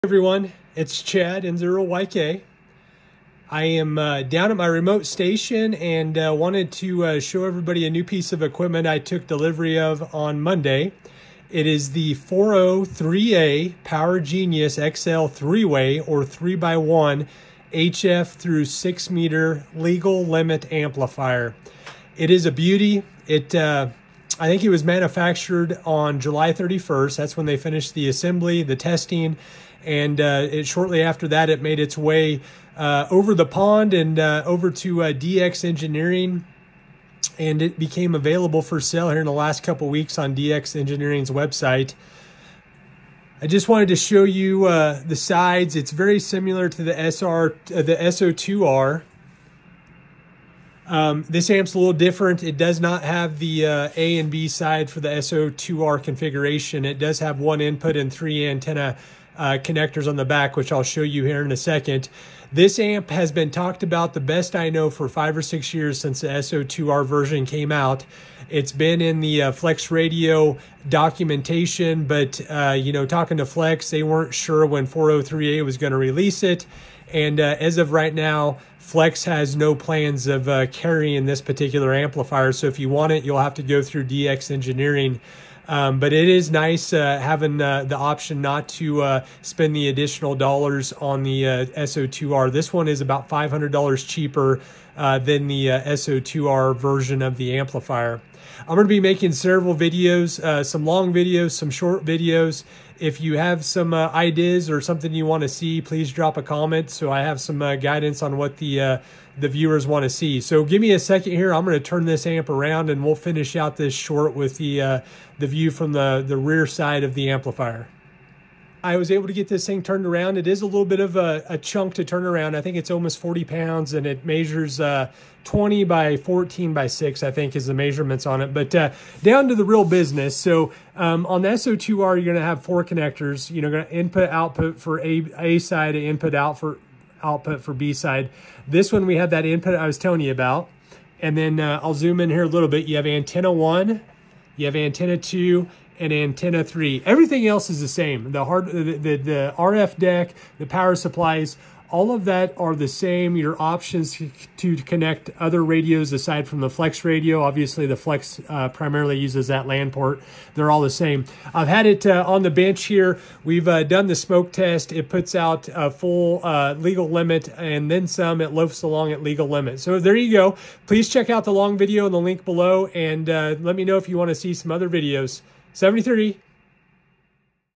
4O3A PGXL 3x1 Amplifier Overview!
Hey Ham Ops!

In this Short video, we'll take a look at the new 4O3A Power Genius XL 3X1 HF/6M Amplifier! This specific amplifier configuration has been discussed for years, but until recently, I do not think it was available, at least in the U.S. market. This video is specific to the unboxing experience; however, over the coming weeks, I'll be posting several more videos covering the operation of the amplifier.

Best 73,
Chad
N0YK